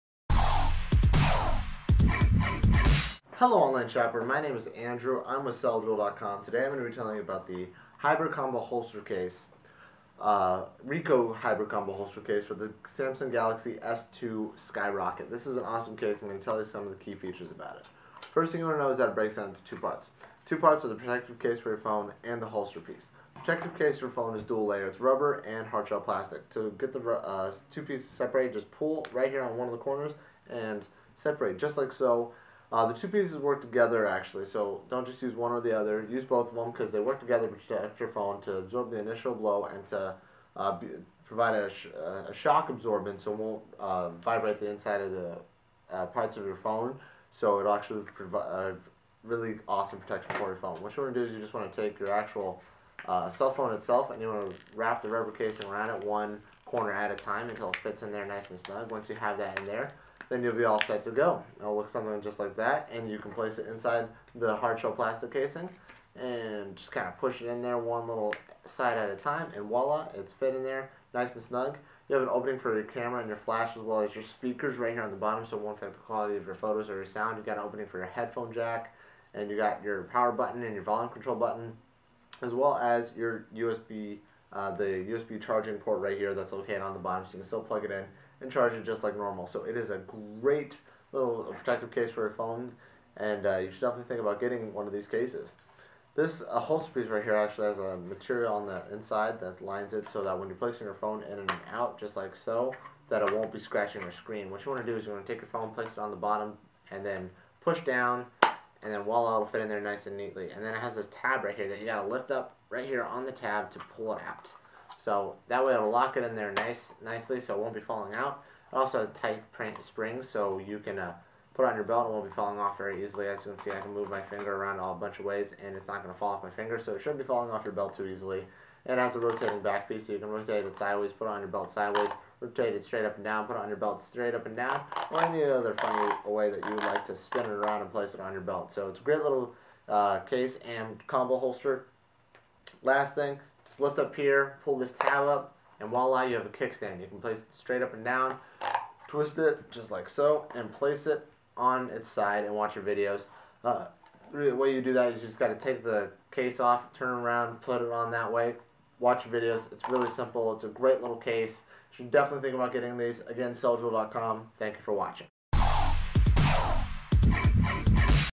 CellJewel.com - Samsung Skyrocket I727 Black on Purple Hybrid Case/Combo
Tired of never finding a case that fits with your holster?  With this Hybrid Case + Combo Holster you get a unique combination of hard and soft materials that gives your phone double protection plus the ease of carrying your phone.  With 180 degrees rotatable belt clip which can function as a kickstand so you can enjoy watching your favorite videos on your phone.